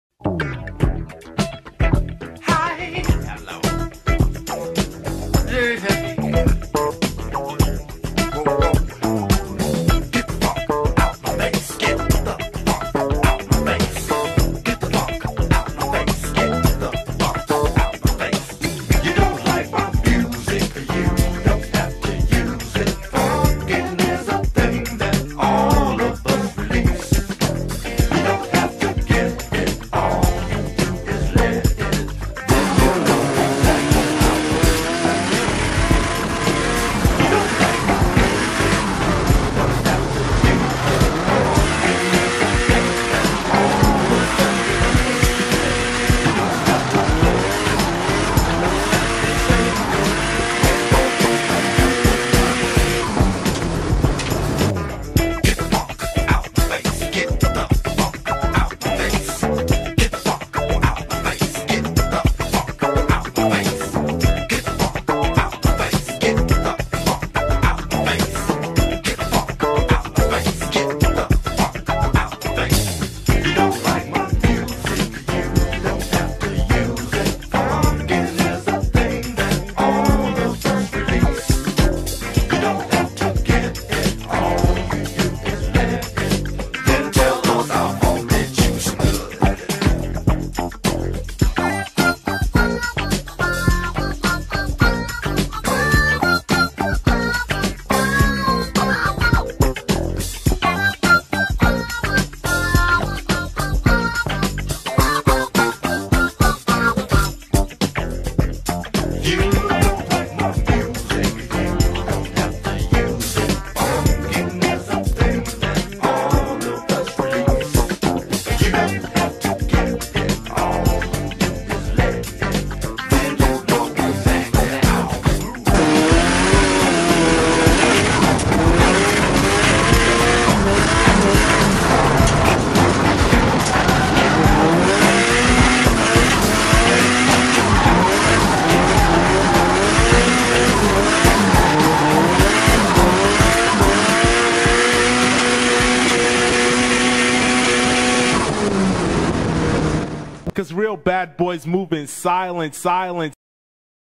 Matt Russell Drift Hustle SUPER8 edition
Here we have some footage of our boy Matt Russell at the World Time Attack Challenge. This time shot on the cutting edge technology of Super 8!